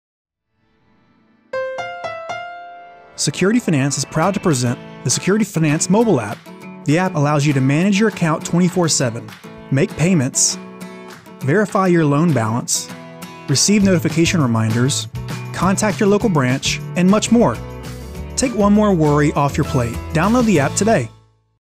Security Finance | Mobile App short | Personal Loans & Taxes
The Security Finance℠ Mobile App makes it easy to:
• Make payments
• Receive notification reminders
• Check your next payment amount & due date
• Verify your loan balance
• View your last payment & amount
• Find a branch near you
• Call to renew your loan
• Contact your local branch easily

Security Finance NMLS 2029421